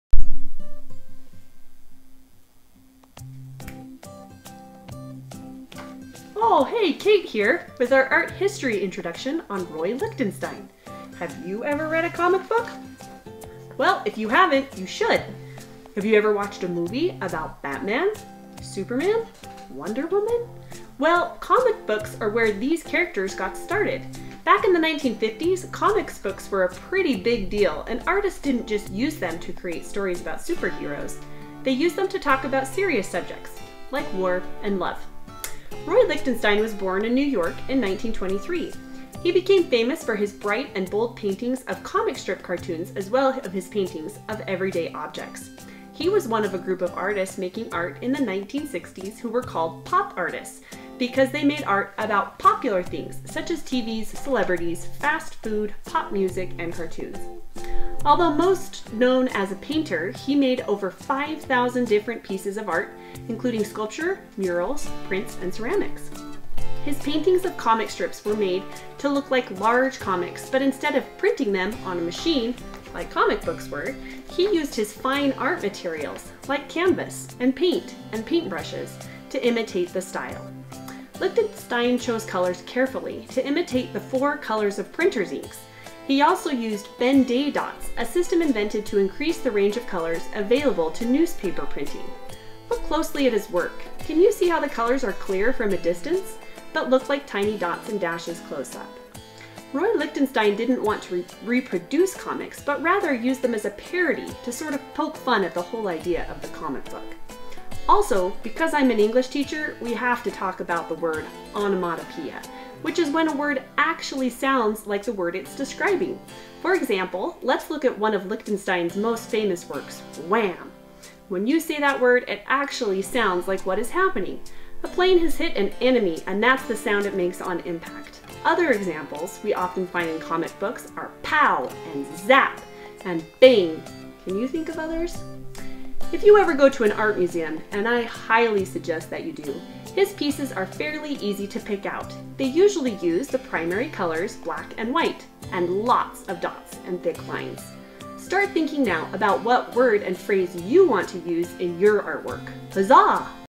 Roy Lichtenstein Art History Introduction
Pop Artist Roy Lichtenstein took comic books to another level. Learn about it here!